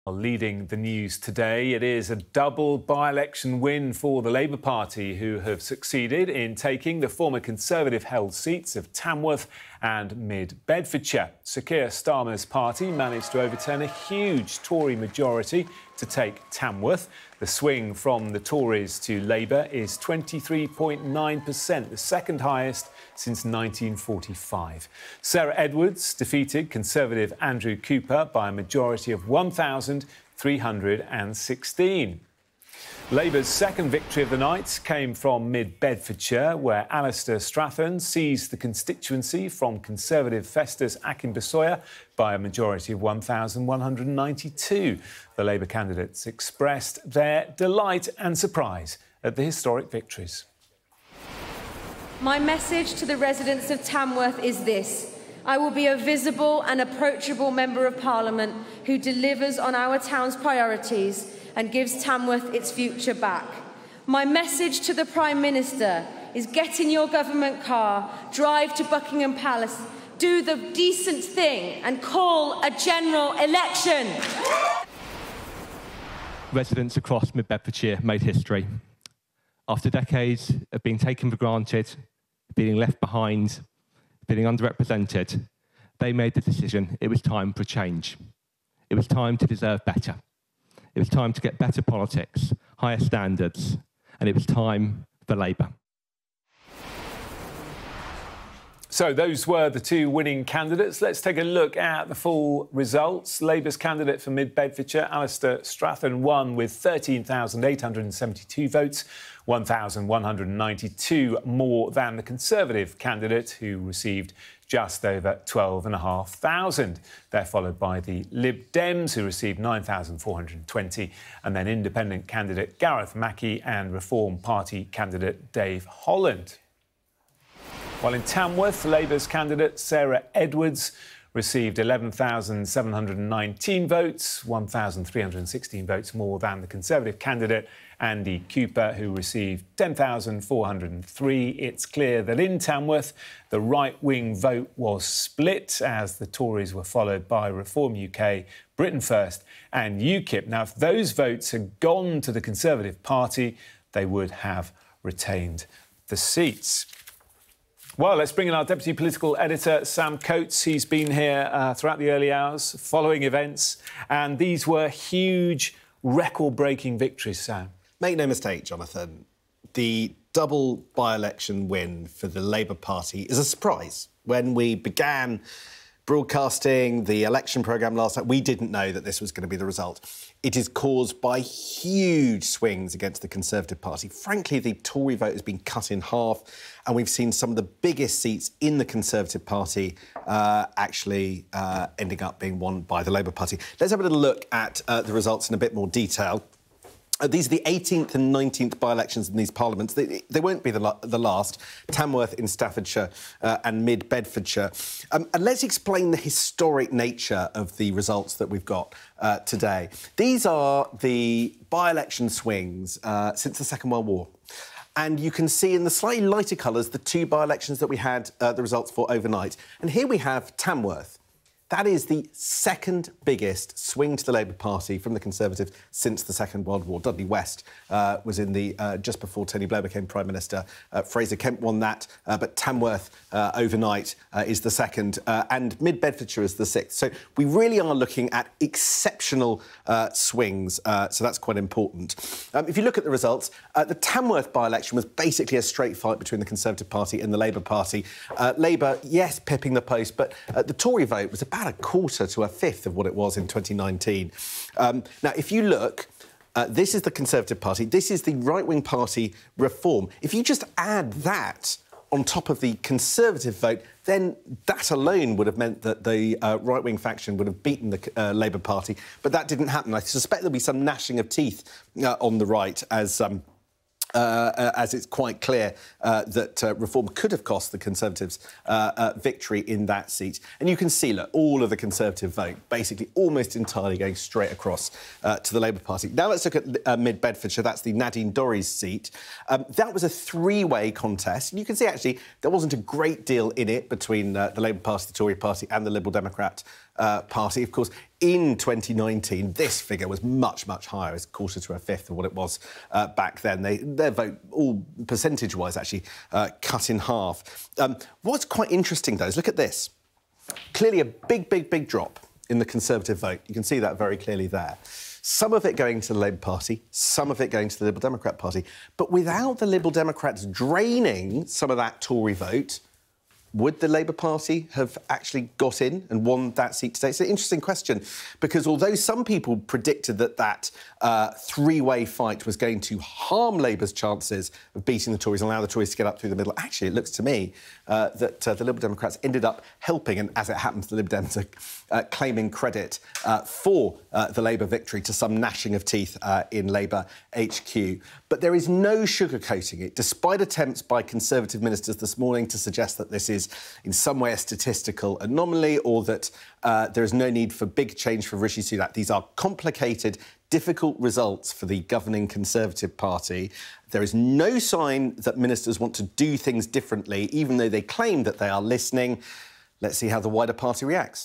Double by-election defeat for Tories as Labour hails political 'earthquake'
Labour have overturned huge Conservative majorities in Tamworth and Mid Bedfordshire to register a pair of stunning by-election victories.